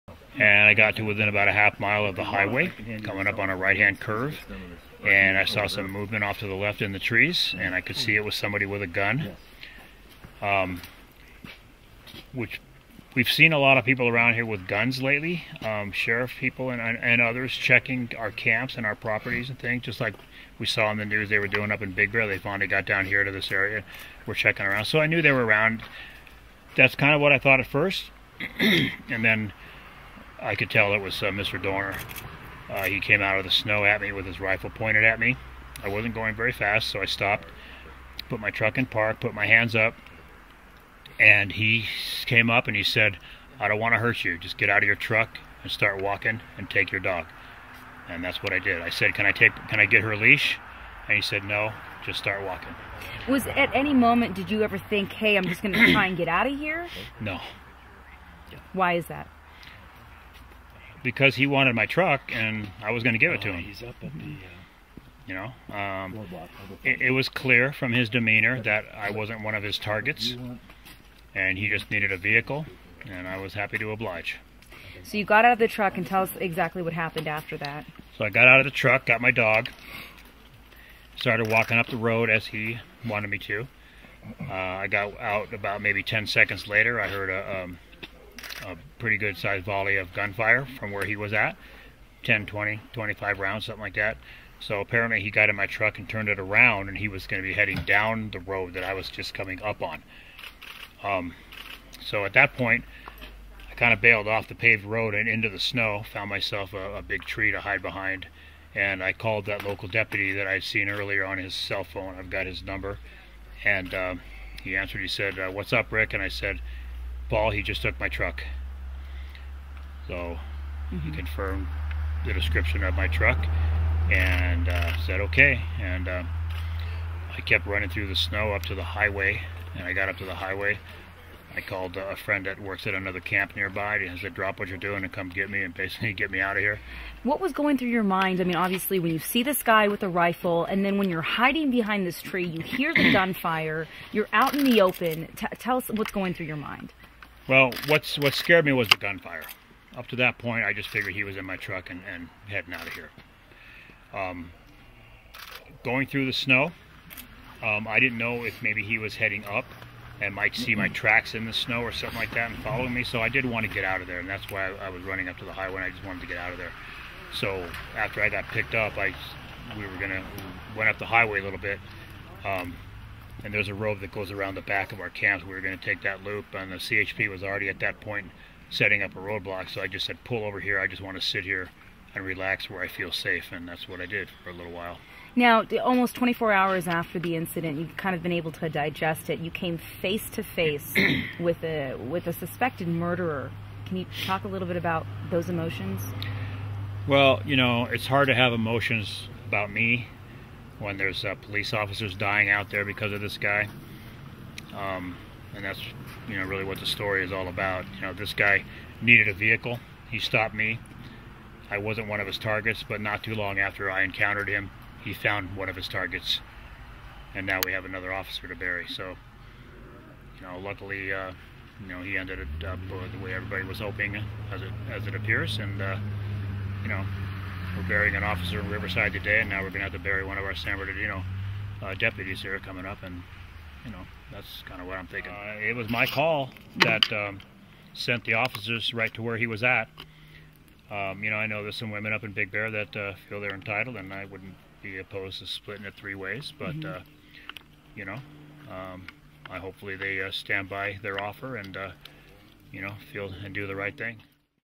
DORNER: Truck hijacking victim describes encounter with Christopher Dorner
Richard Heltebrake, who was carjacked in the San Bernardino Mountains by fugitive Christopher Dorner, describes his encounter with Dorner just before Tuesday's gunfight.